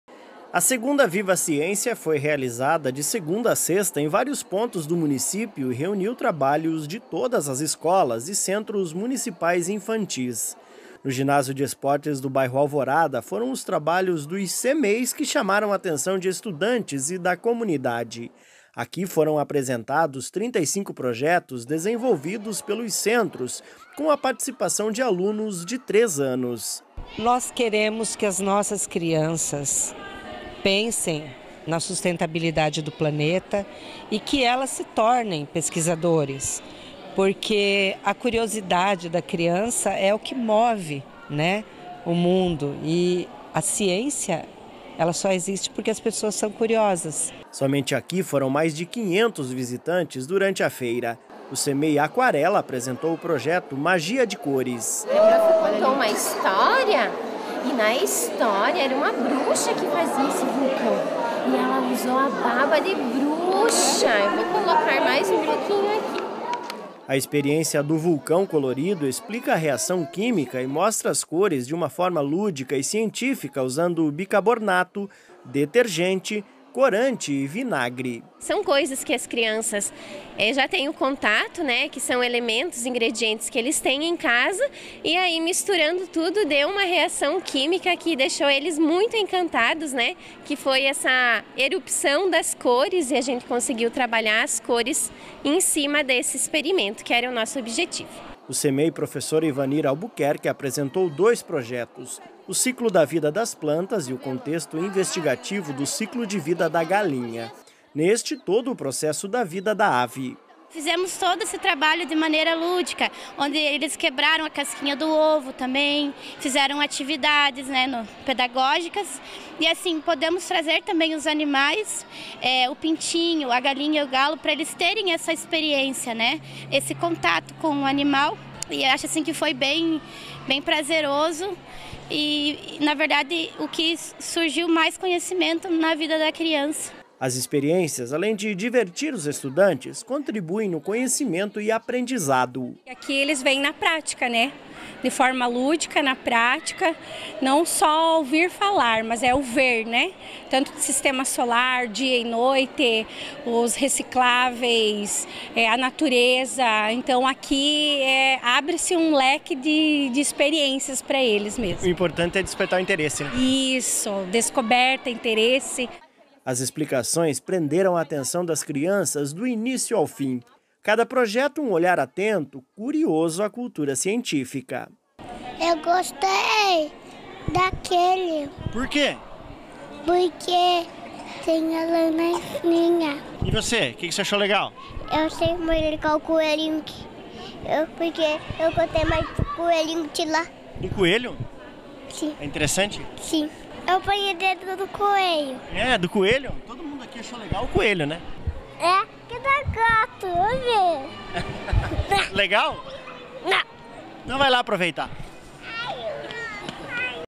Secretaria de Educação de Beltrão realiza 2ª Viva Ciência
Educação: A feira foi desenvolvida por estudantes da rede municipal de educação.